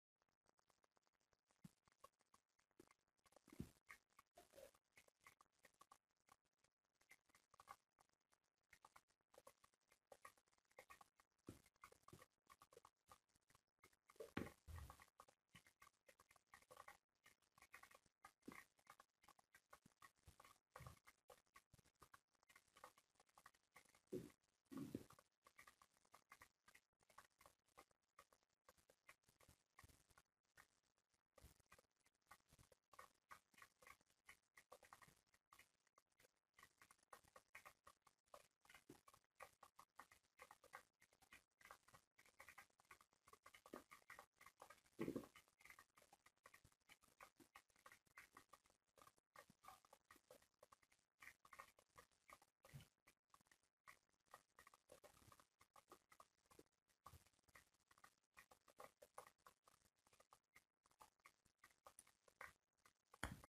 Bio свічка камін. Bio candle fireplace. Bio свечка камин.
Oil candle with the sound of a fireplace on a wooden wick how to make a link to the video.